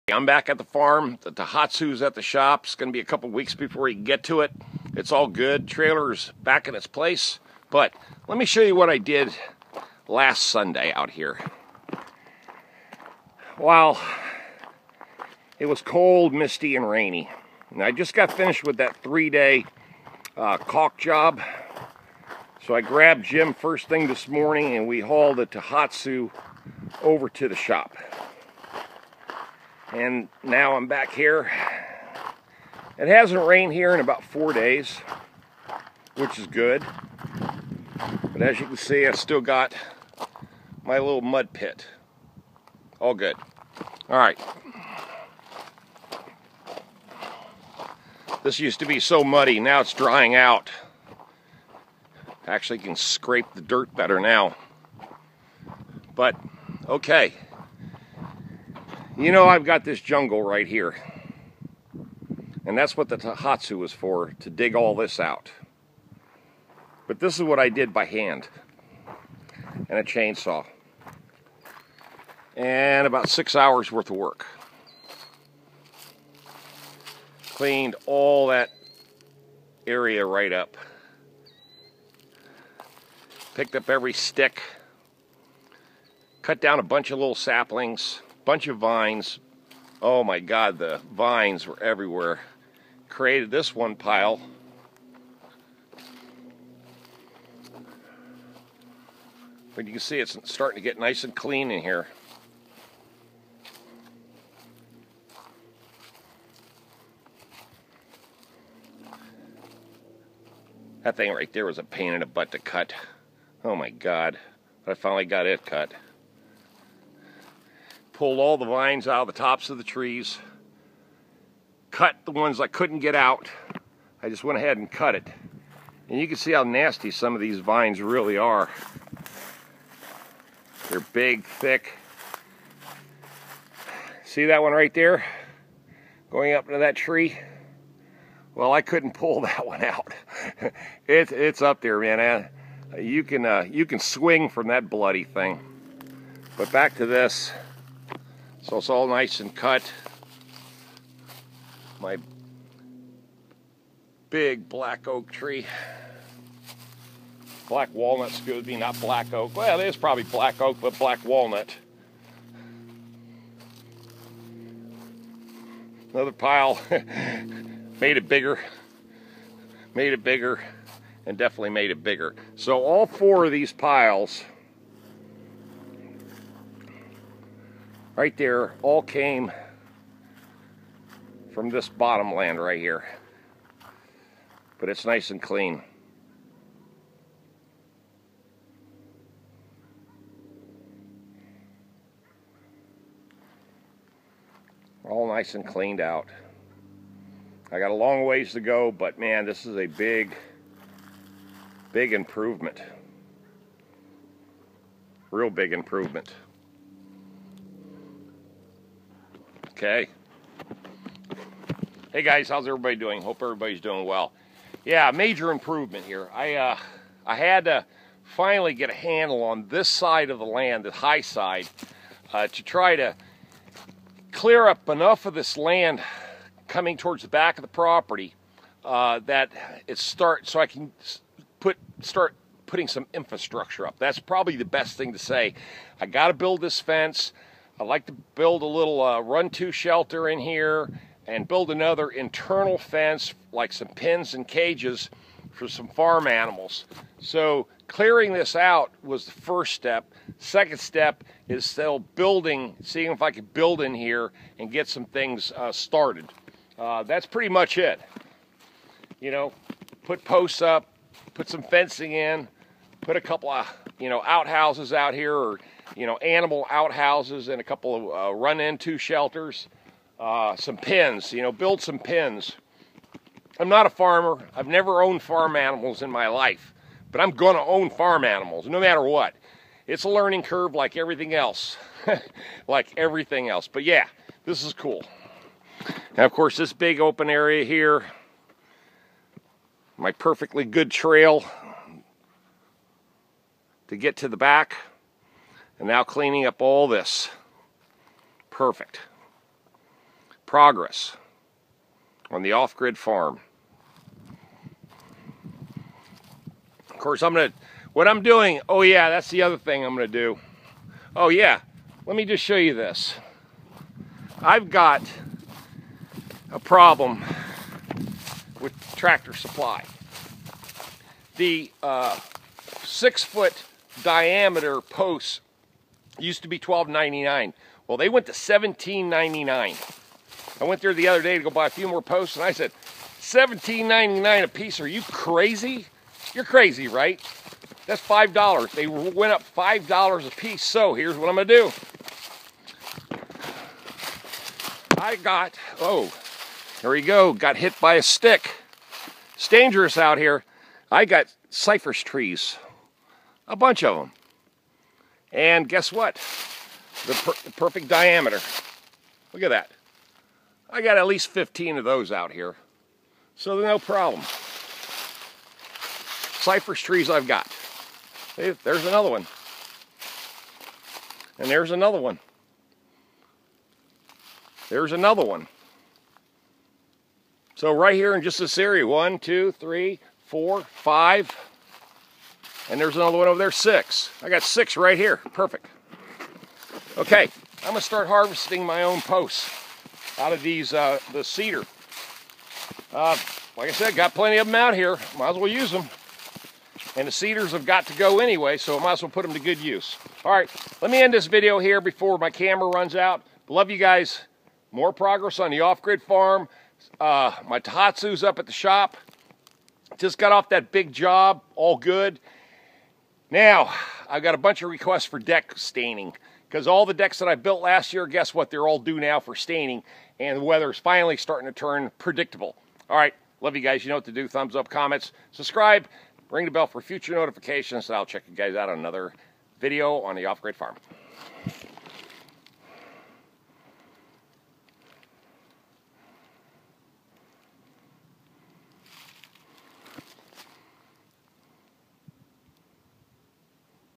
CLEARING MORE LAND. OFF GRID FARM.
HI GUYS JUST AN UPDATE VIDEO, ON HOW THE LAND CLEAN UP IS COMING ALONG. ENJOY! ON THE OFF GRID FARM.